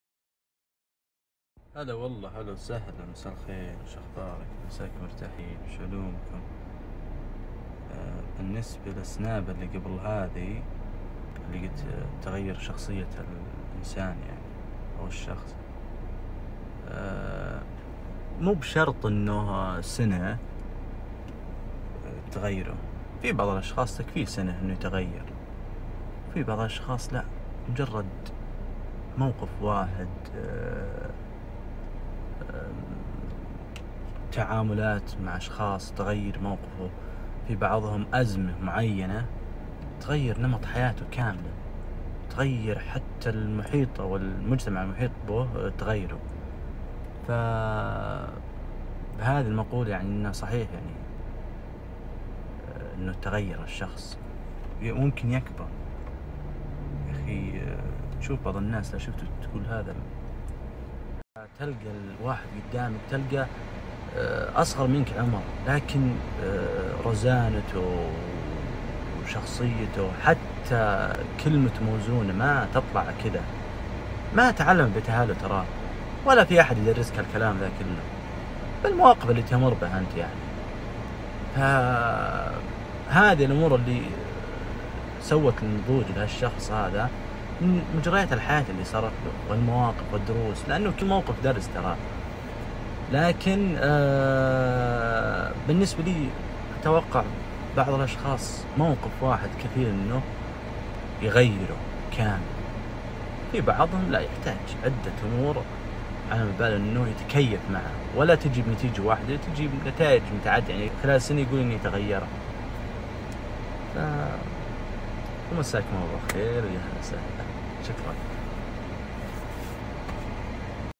هل ممكن تتغير الشخصية | عبدالعزيز القفاري
سنة واحدة فقط قد تمر على الانسان تتبدل فيها أفكاره وشخصيته ويستغني عن أشياء عديدة ويشعر أنه كبُر في عمره لسنوات كثيرة. وأزداد وعياً وحكمة وأخذ ينظر إلى الحياة والناس بشكل أوضح وأعمق وواقعية بعيداًعن المجاملات وأخذ يضع كل شيء بمكانه الصحيح فيعطي من يستحق ويبعد من لا يستحق ويسلك السبل والطرق التي تجعله أكثر حرصاً وحيطة من أن يقع في أخطاء وعثرات الماضي حتي لا يتألم مرة أخري